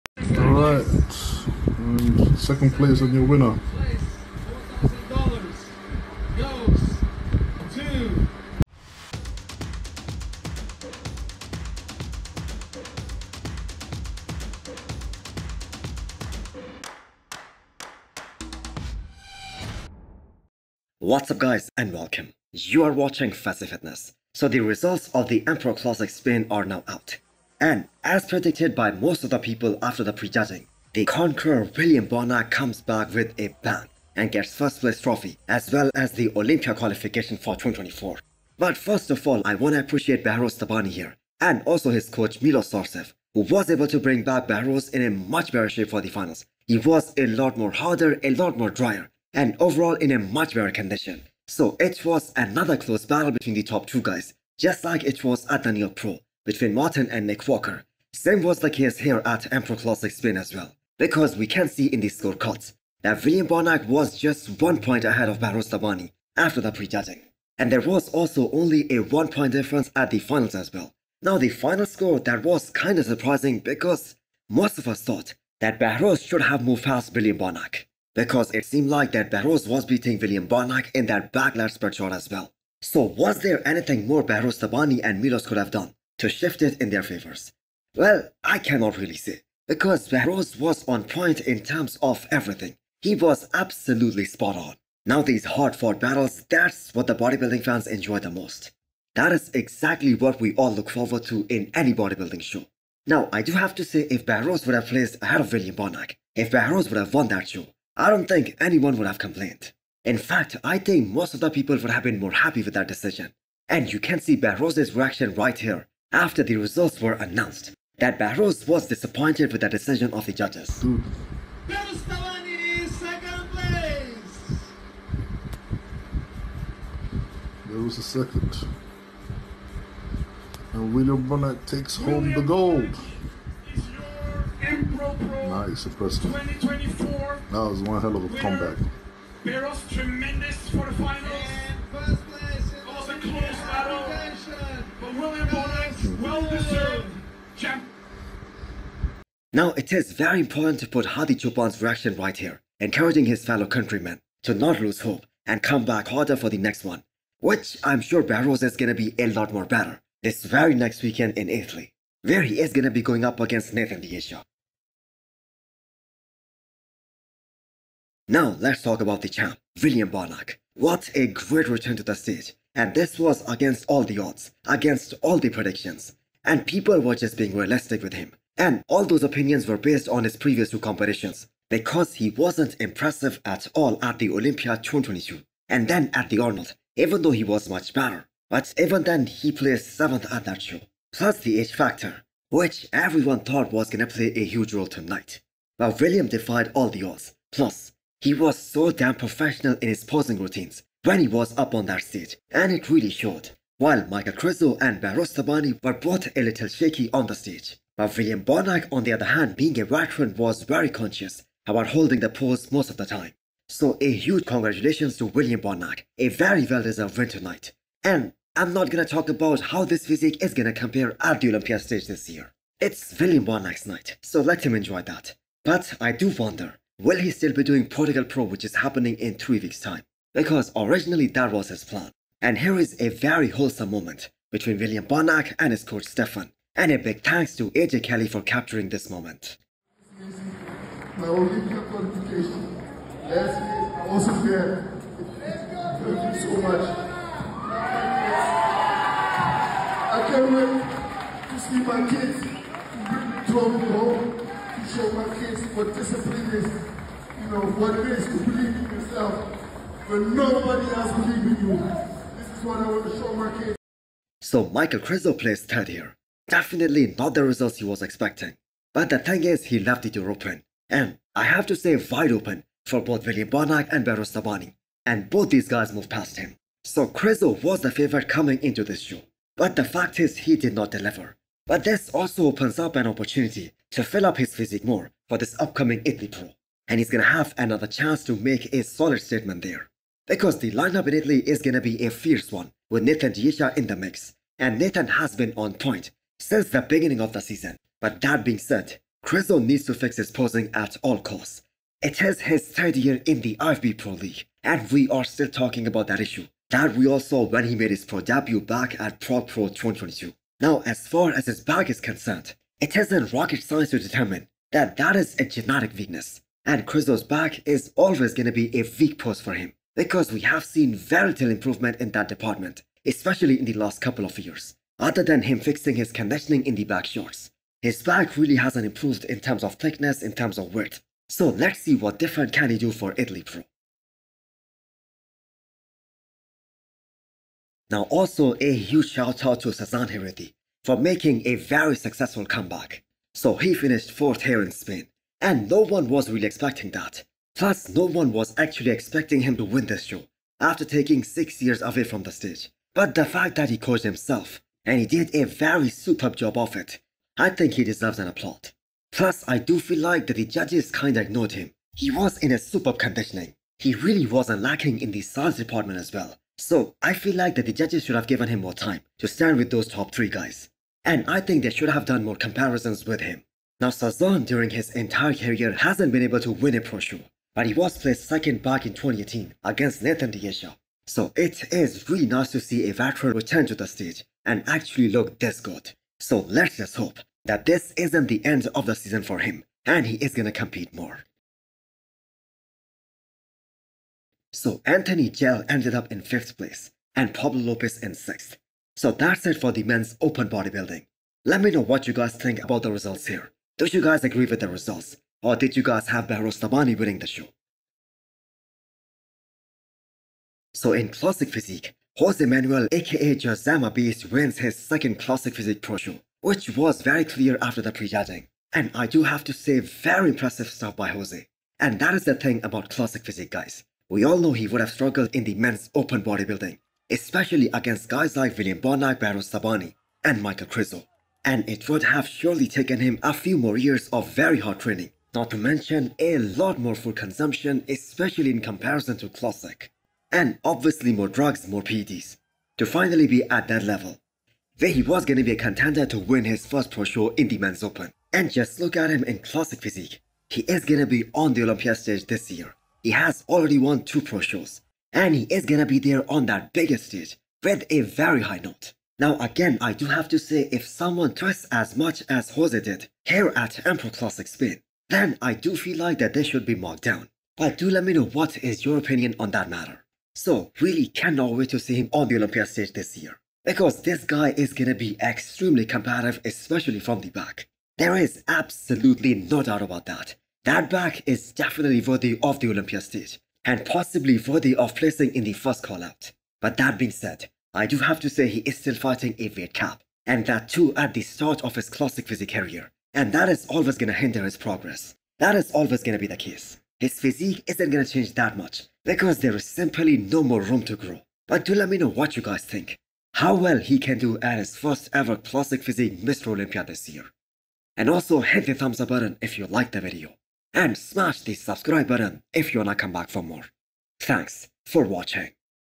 Empro Classic Spain 2024 RESULTS COMPARISONS & ANALYSIS + Was Behrouz robbed ? Krizo's liability
Empro Classic Spain 2024 RESULTS COMPARISONS & ANALYSIS + Was Behrouz robbed ? Krizo's liability , Classic Physique results , Sasan's successful comeback

Code :rogier25